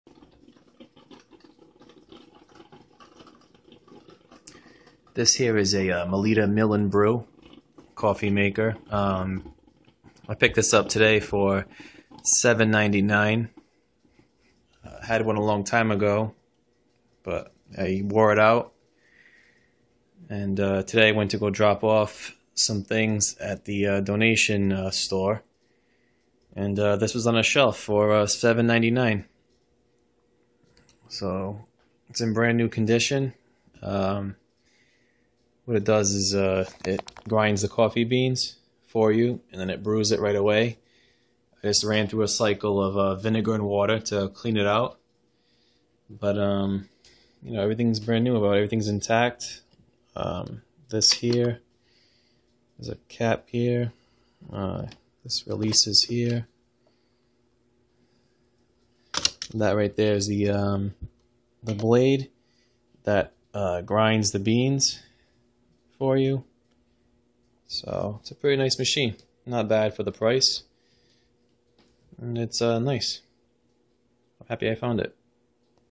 Melitta mill & brew coffee maker
Melitta coffee maker that mills and brews. Throw in some coffee beans and the beans are ground in the filter itself. Very nice machine